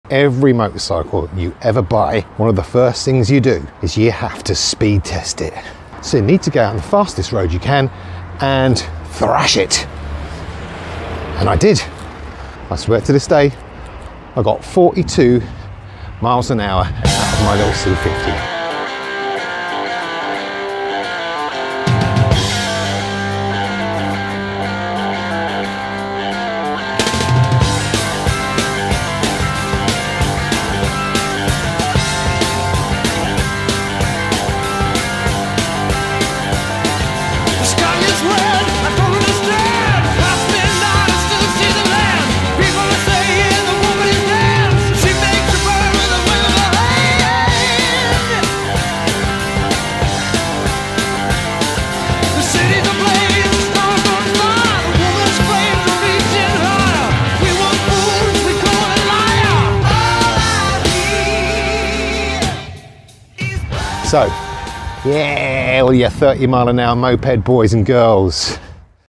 Honda C50. The most popular bike in the world.
Brite Times presents The Honda C50. The most popular bike in the world.
Honda's, Yamaha's, Suzuki's, Kawasaki's, KTM's, Triumph's, I've owned them all.
So in 1982 I managed to get my hands on a Honda C50.
It was a dream come true.
A different style but we'll be back to the other format next episode.